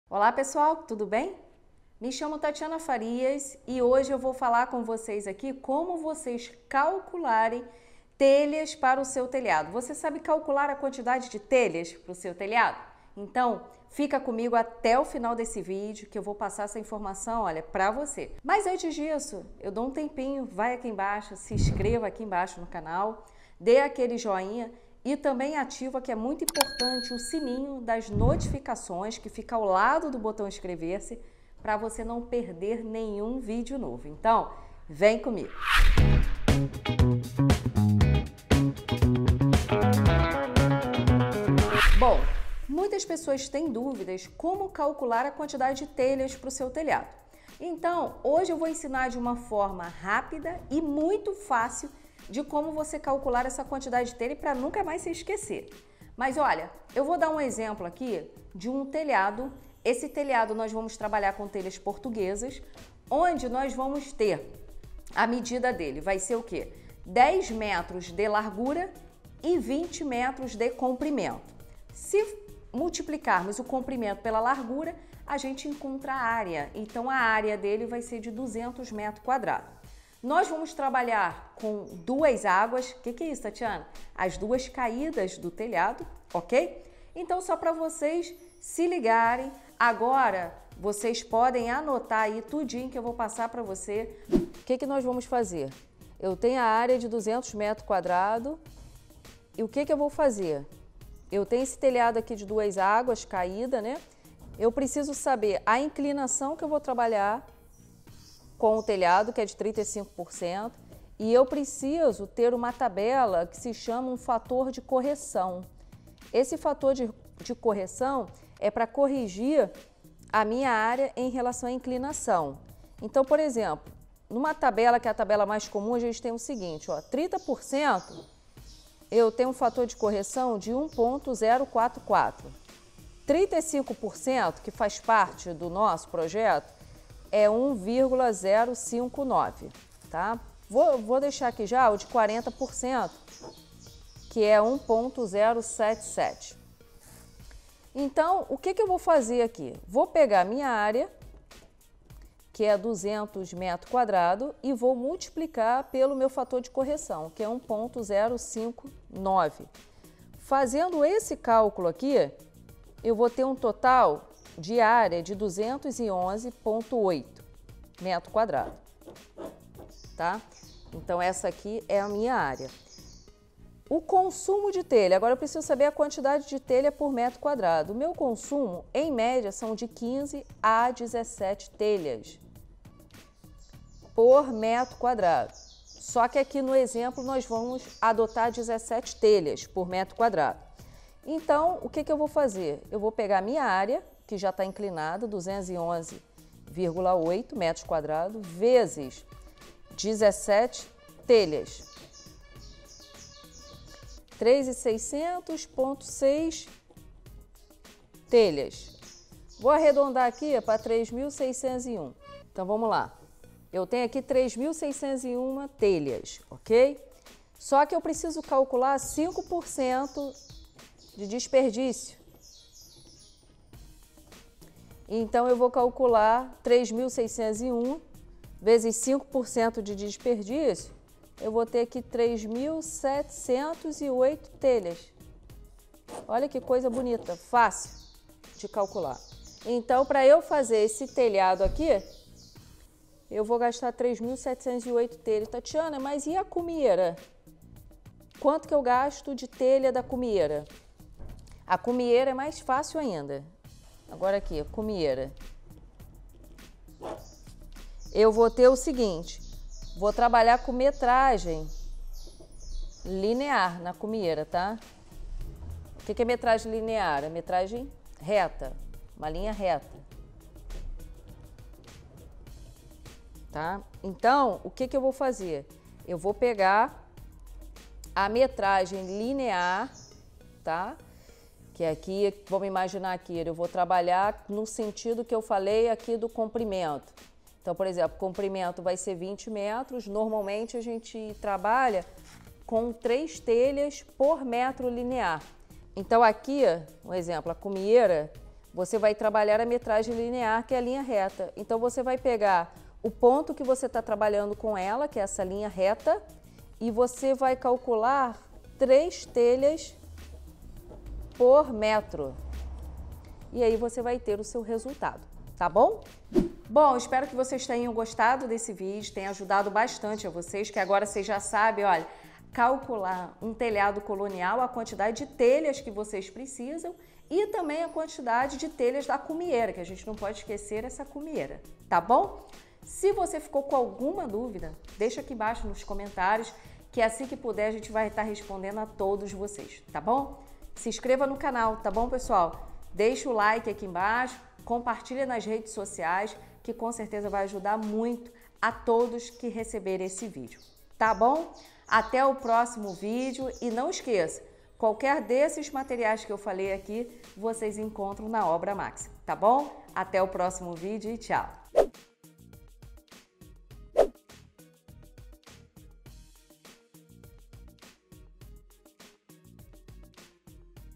Como calcular a quantidade de telhas passo a passo
Aprenda de forma fácil como fazer o cálculo de quantidade de telhas portuguesas neste passo a passo imperdível com a @engenhariafeminina !
Para começar, devemos descobrir a área total do telhado. Para isso, multiplicamos a largura pelo comprimento. Neste exemplo, temos um telhado de telhas portuguesas, 2 águas, 20m por 10m, com inclinação de 35%.

O cálculo da área é: 20m multiplicado por 10m, temos área total de 200m².

Um ponto importante é o fator de correção. Ele é indicado conforme a inclinação do telhado e será multiplicado pela área total. Os principais fatores são:

Inclinação 30%: 1,044
Inclinação 35%: 1,059
Inclinação 40%: 1,077

Fazendo a conta, temos 200m² multiplicado por 1,059, que é igual a 211,8m².

Geralmente, o consumo de telhas é entre 15 a 17 telhas por metro quadrado, a depender do fabricante. Nesse caso, temos 211,8m² multiplicado por 17, resultado = 3.600,6, arredondado, temos 3.601 telhas.

No entanto, é sempre importante considerar uma margem de 5% para quebras, reparos e desperdício. Dessa forma, neste telhado de telhas portuguesas, iremos precisar de 3.708 telhas.

Outro ponto a ser calculado é a quantidade necessária de cumeeiras. Neste caso, basta multiplicar o metro linear, isso é, em linha reta no sentido que serão instaladas, pela indicação do fabricante, geralmente, 3 telhas por metro.